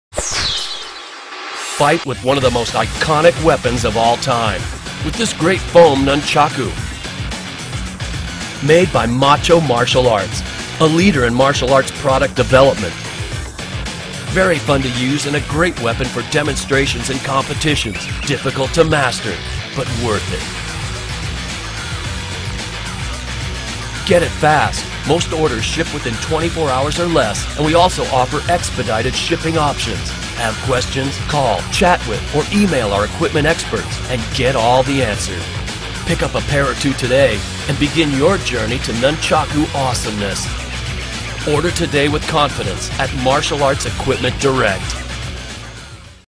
Macho Rubber Foam Nunchaku - MAEQD.com
Product Description:
Foam padded chucks specially designed to help you train safely and
comfortably at full force.

Features:
* Made with a traditional cord attachment for ease of movement on lighter
weight design.
* Constructed with 1/4" foam padding thick and extends the length of each
handle.
Important Note:
Due to state laws, we are can not ship this item to the following states:
CA, MA,NY, AZ and RI.

Customer Reviews:
4 of 5 Stars: Good for practice. - David from Arizona.